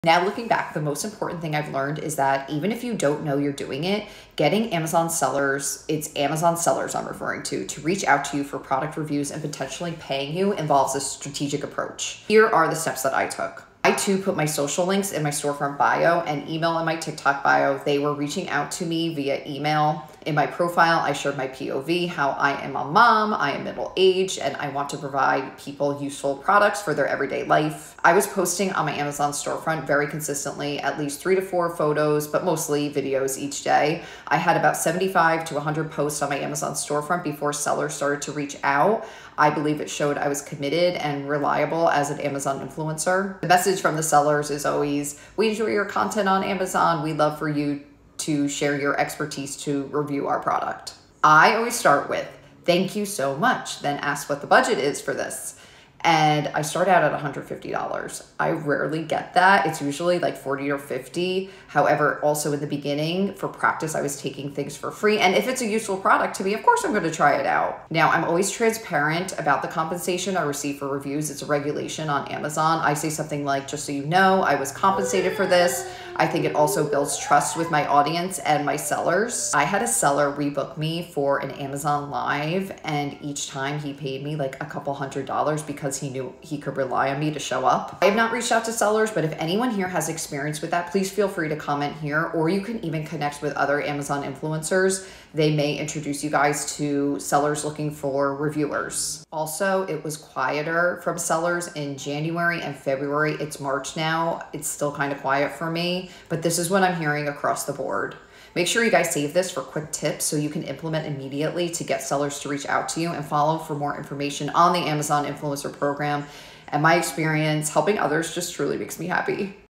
Unlocking the Power of Product Reviews as an Amazon Influencer: How I Earned Almost $1K!
In this video, I'll unveil the secrets behind earning nearly $1K by reviewing products. Join me as I break down actionable steps, revealing the exact strategies and methods I've used to monetize my reviews. From selecting the right products to review to maximizing your earnings potential, I'll share practical tips and insights that you can implement today. Get ready to turn your passion for product reviews into a lucrative side hustle!